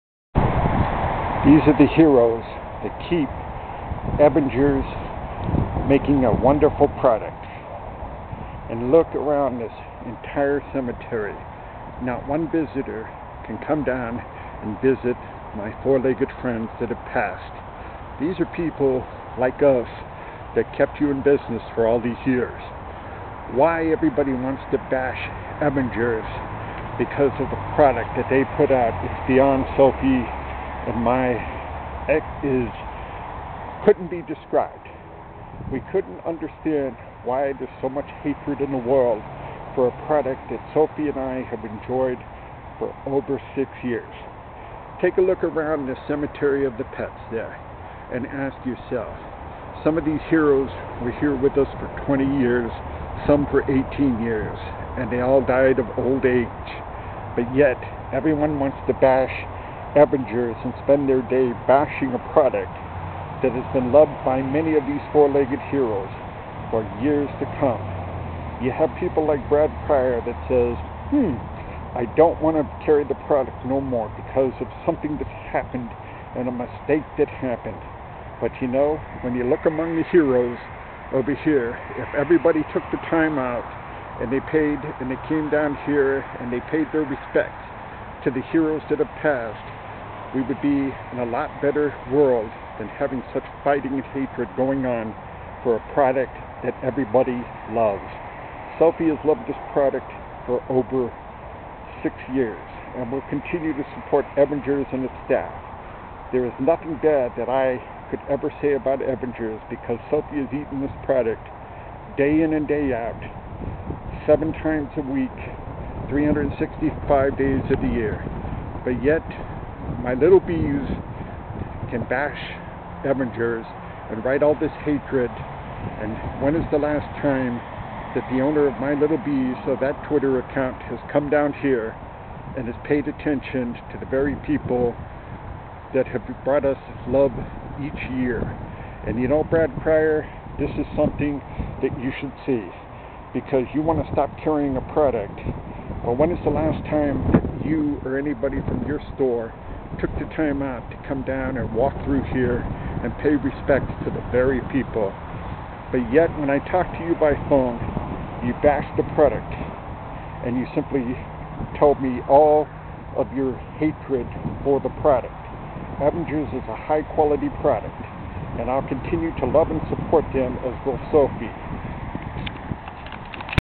100 3666
We Can never have Enough Evanager's Hunk Of Beef. Why would "any Pet Store Want To Hurt Sophie ? By Not carrying Evanager's Hunk Of Beef is beyond Me.